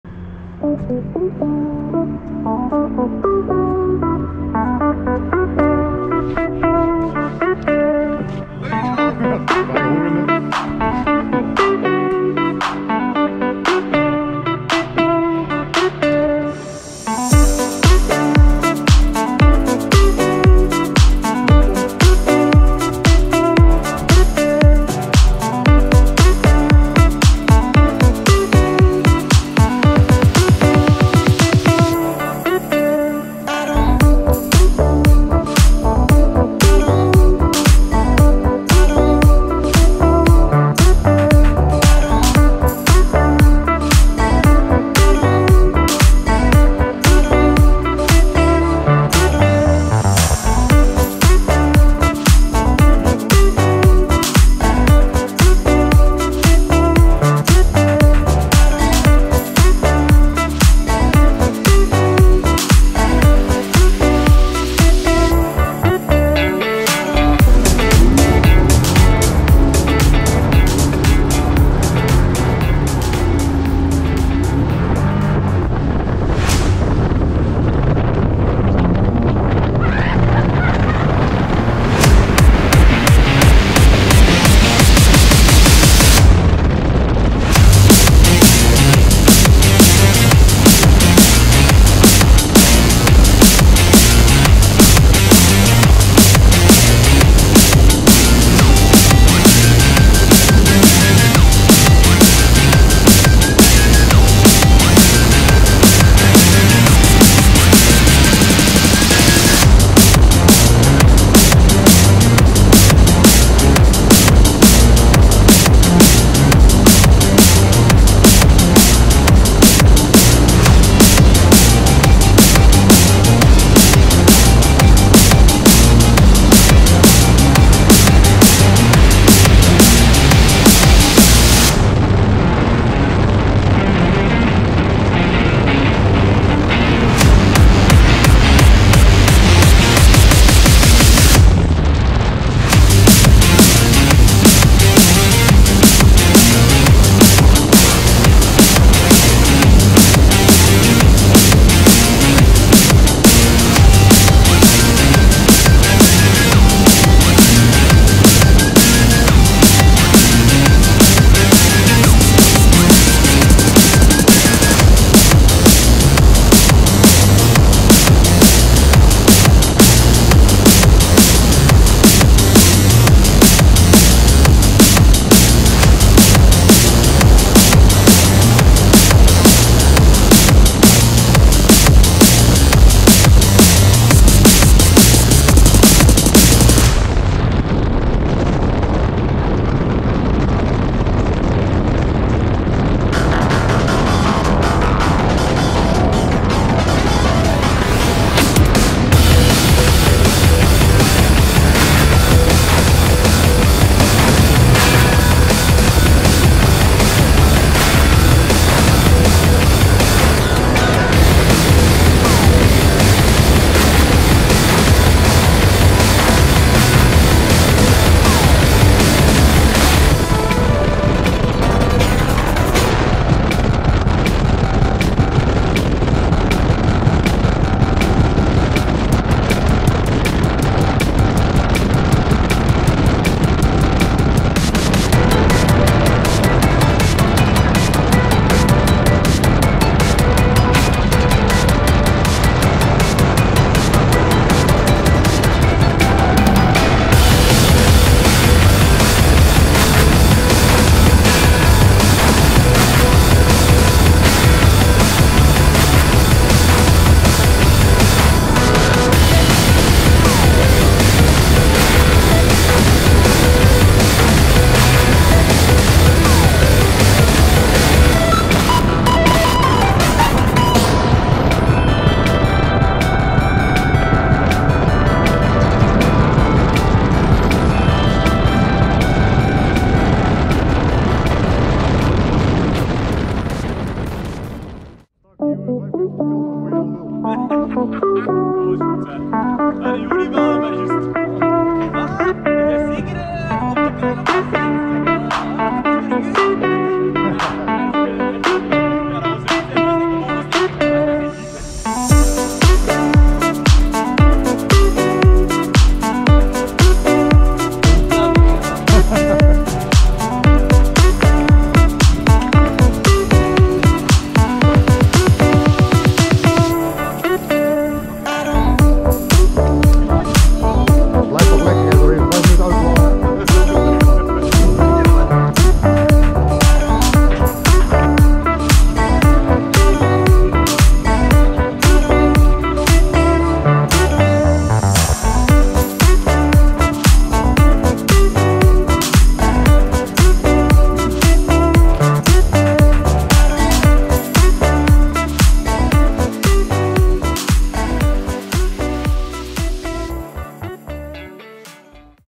SpeedbootjesZondag met Piratensap afsluiter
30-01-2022 zondag vaartripje, helaas geen film van alle aanwezigen :(
Bernico, Phantom, Ring, Optimax, ProXS, XR2, Evinrude